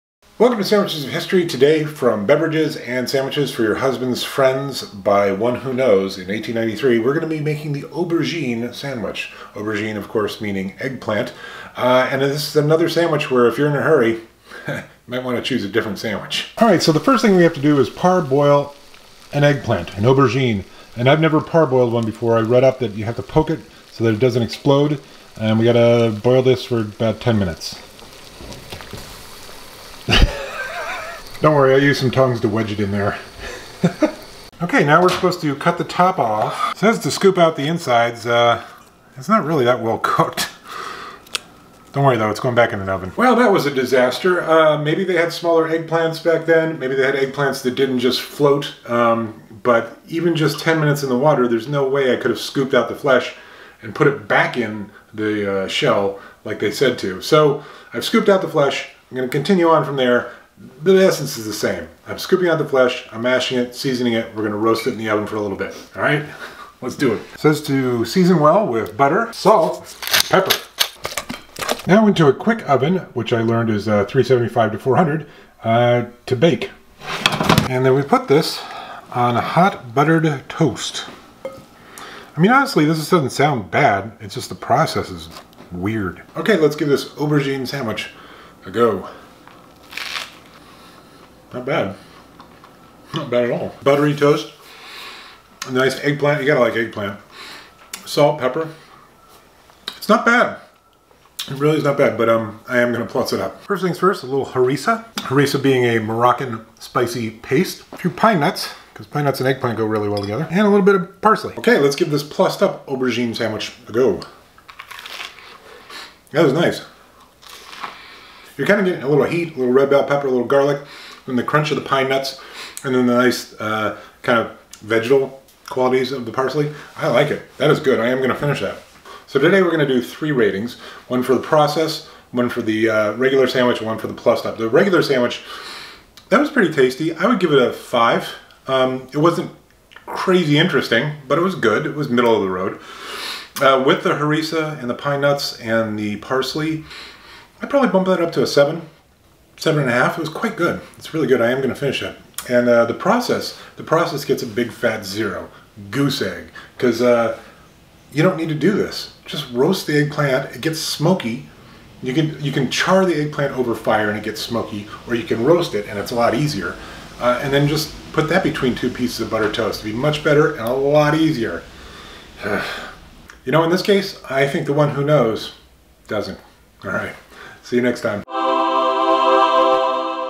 Aubergine Sandwich (1893) on Sandwiches of History
The Aubergine (eggplant) Sandwich from 1893’s “Beverages and Sandwiches for Your Husband’s Friends” by “One Who Knows” is an odd one. Not so much for the ingredients nor the resulting sandwich. But for the process.

Get sneak peeks, exclusive vids, merch, and monthly livestreams, and more!